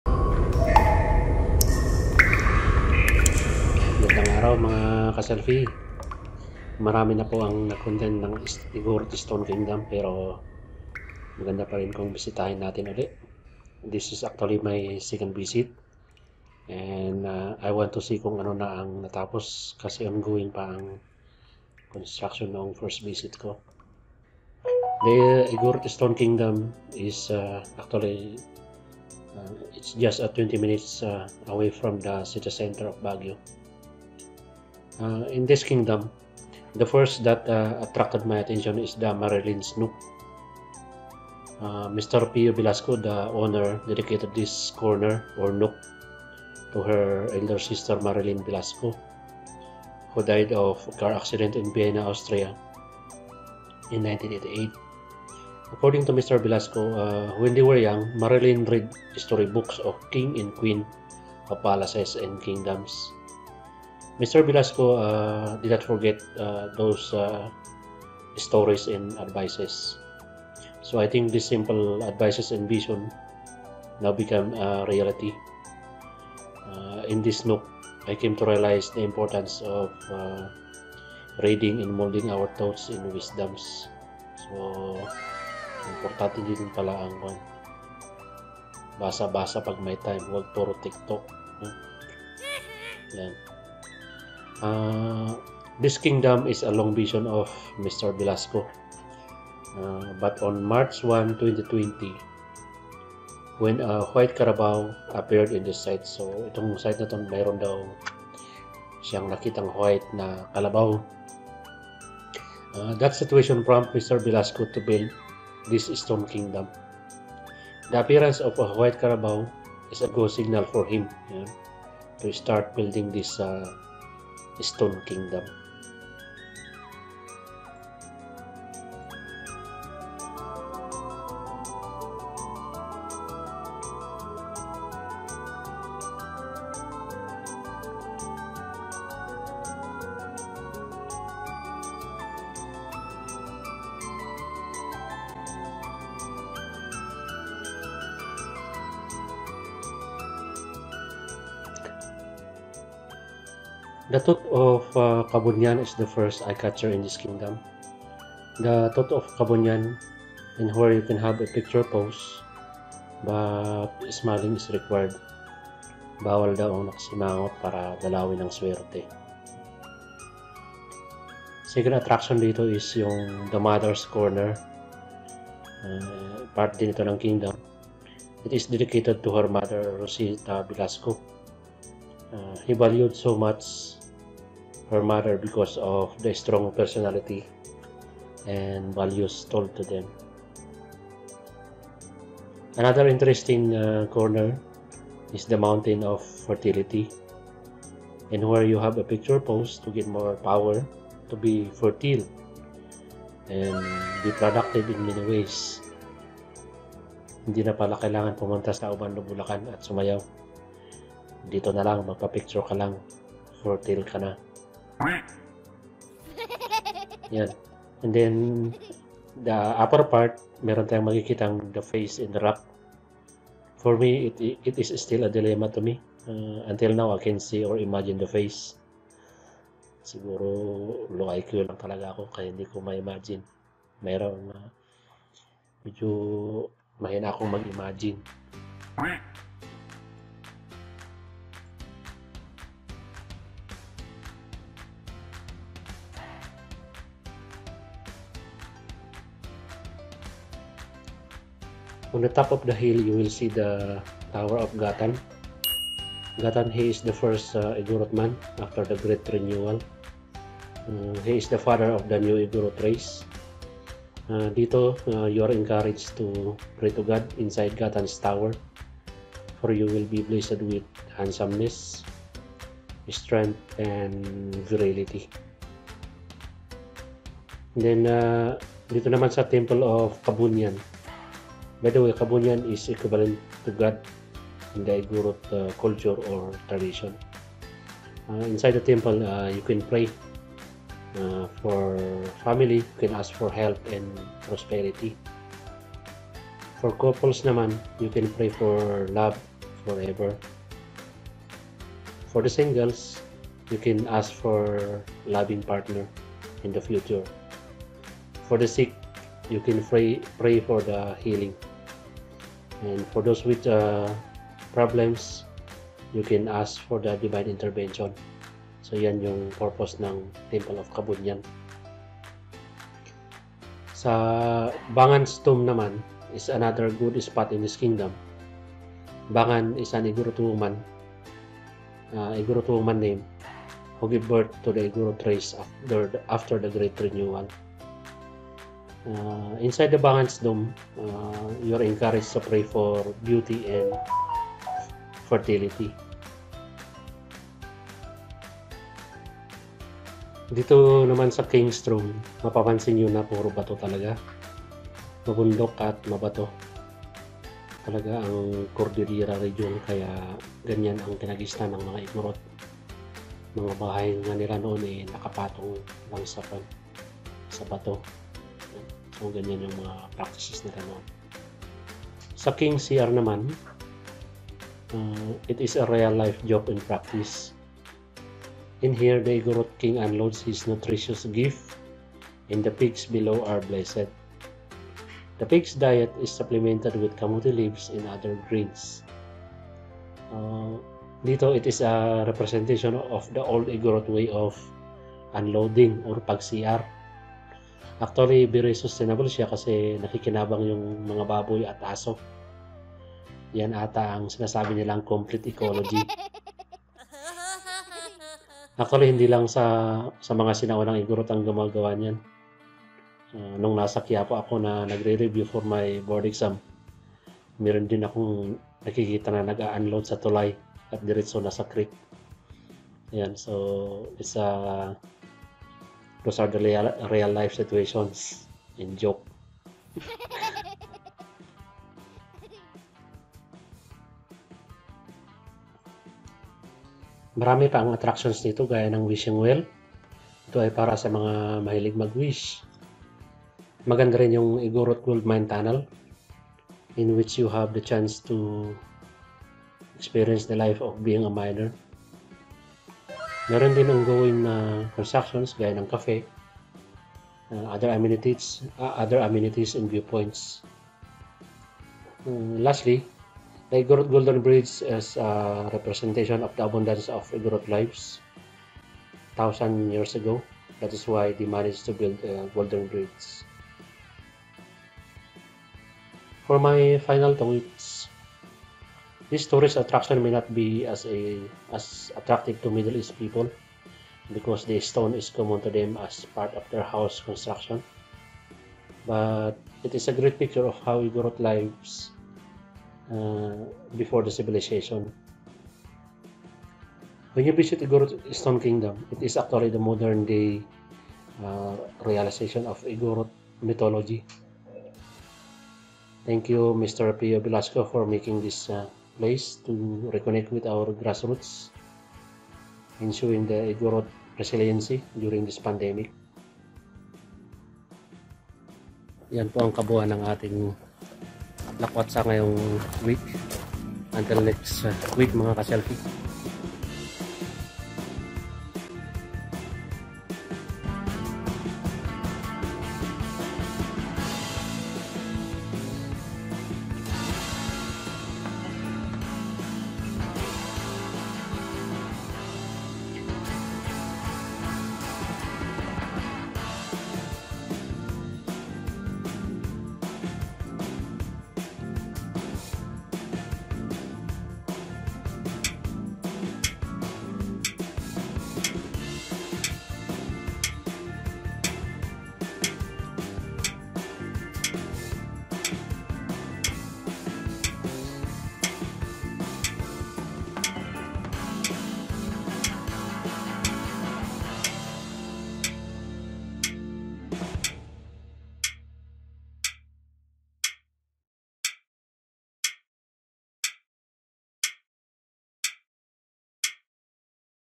Igorot Stone Kingdom of Baguio City, Philippines (Vlog#8)
Exploring the realization of Igorot mythology. The new tourist attraction and ongoing construction of Baguio City.